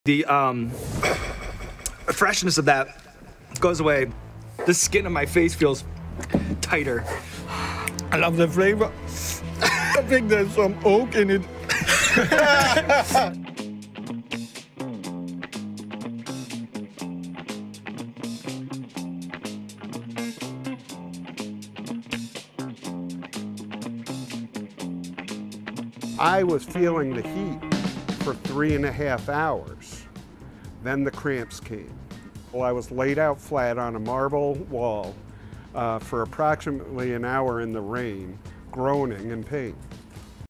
"And then the cramps came": Man eats world's hottest chilli
Pepper X has been crowned the hottest chilli in the world by Guinness World Records and the man that created it describes what it was like eating one whole.